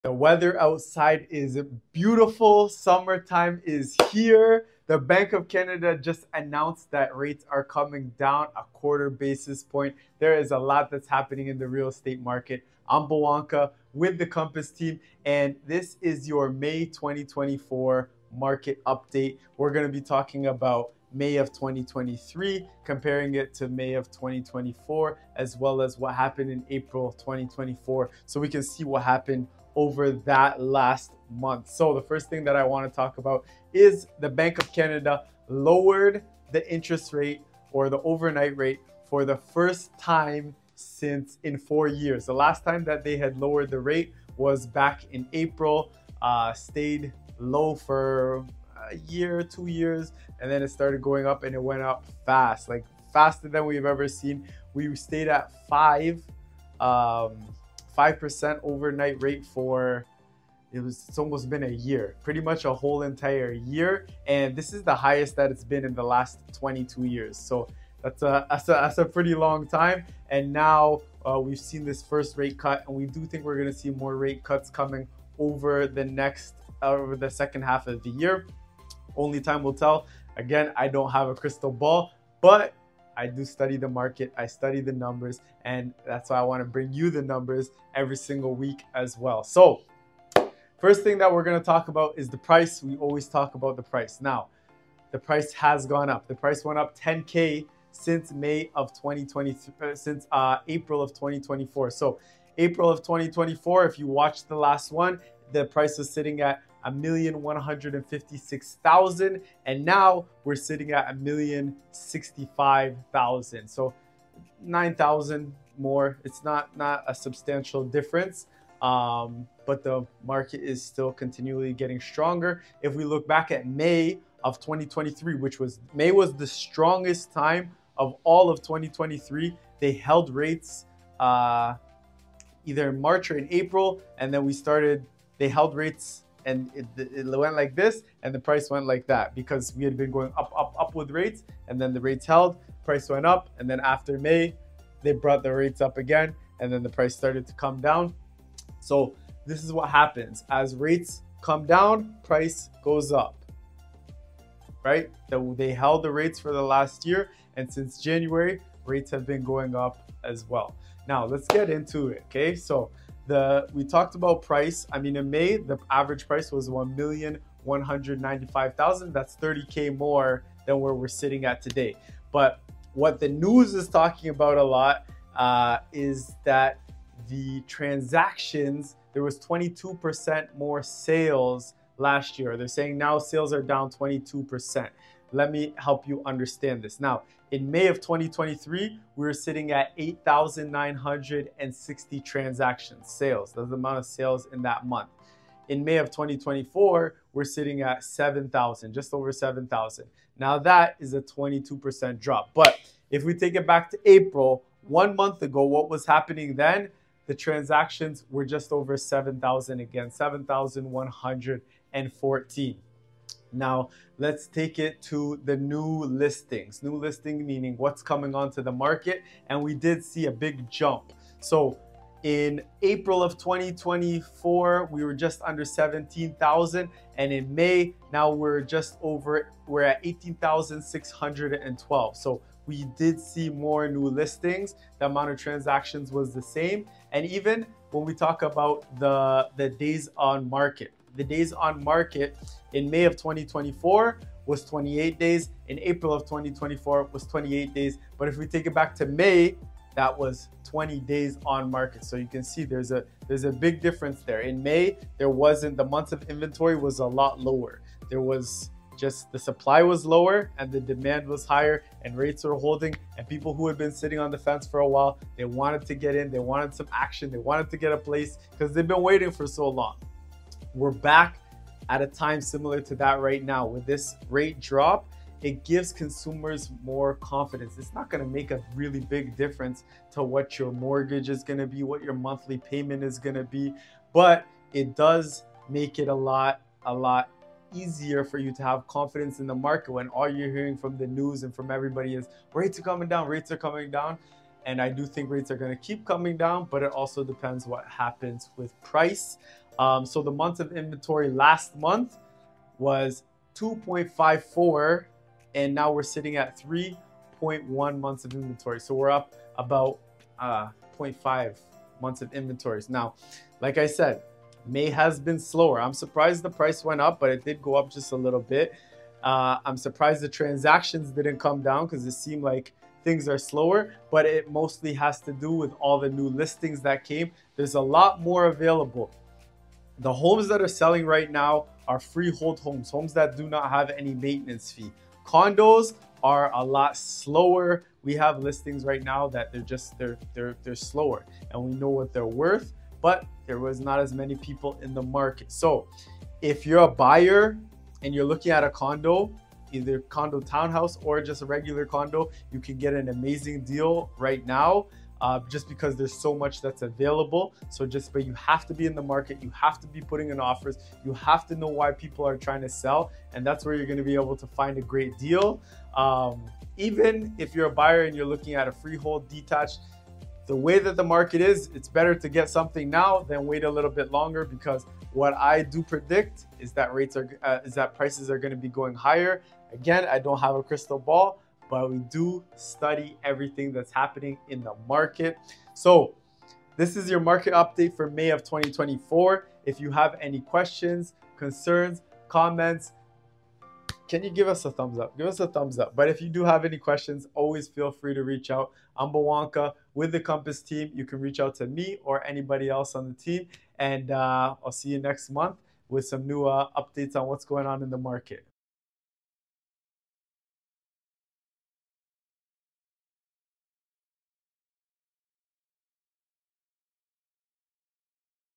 Bank of Canada Lowers Rate For First Time in 4 Years
P.S. Whenever you’re ready we can connect.

The May Market Update 2024 highlights significant shifts in the Toronto real estate market. The Bank of Canada (BoC) has implemented a rate drop, lowering the interest rate by a quarter basis point for the first time in four years. This BoC rate drop is expected to boost housing demand and influence future property investment trends.

In terms of housing statistics, the average home price in the Greater Toronto Area (GTA) saw a slight increase of $9,000 from April to May 2024, reaching $1,165,000. However, this is still $30,000 lower than the prices observed in May 2023. The housing inventory has expanded, with new listings rising from just under 17,000 in April 2024 to 18,617 in May 2024. Despite this increase in listings, the sales transactions experienced a 22% drop compared to the previous year, maintaining levels similar to April 2024.

The market analysis indicates that the Toronto housing market is seeing a longer average time on the market, with properties now staying on for 28 days compared to 20 days in May 2023. This trend suggests a more cautious buyer environment, influenced by the fluctuating interest rates.

The real estate market forecast suggests that while the rate drop has given a slight boost to housing prices, the market remains balanced with increased inventory. For those interested in real estate investing, especially in the Toronto real estate market, now might be an optimal time to consider property investment, given the availability of properties and potential for good deals, particularly in the condo segment.

Overall, the Toronto real estate market update for May 2024 reflects a period of adjustment following the BoC's rate cut, with market trends indicating a cautious but potentially rewarding environment for buyers and investors.